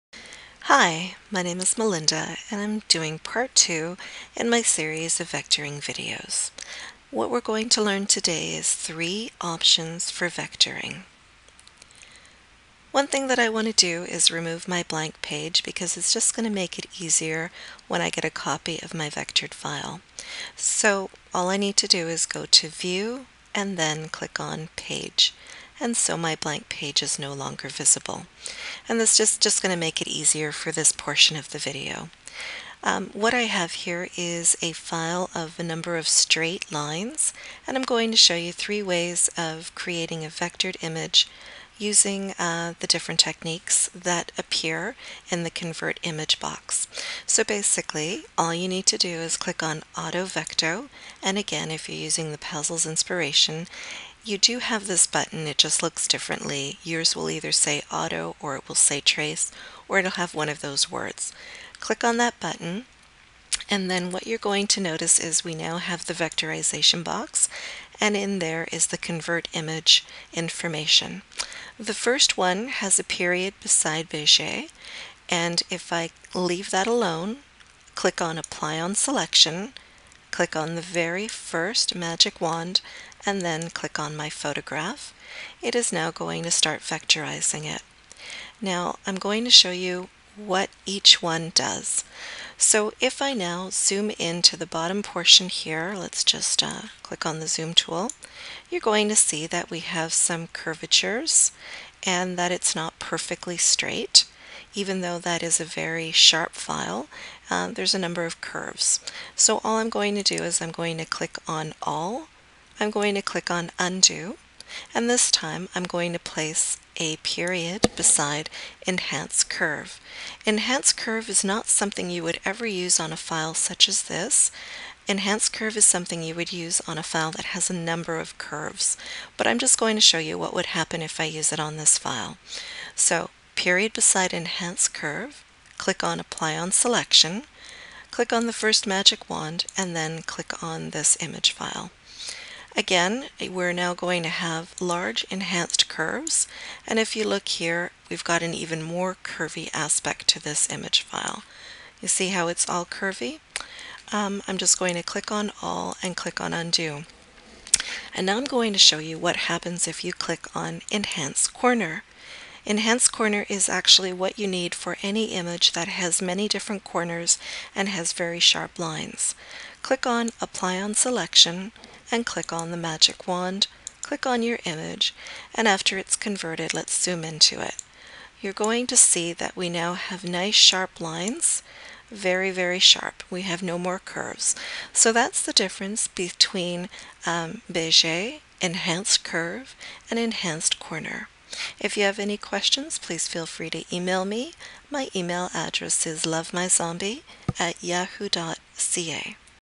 Funtime. Part 2) Three Options for Vectoring
Three options for vectoring in the Funtime software.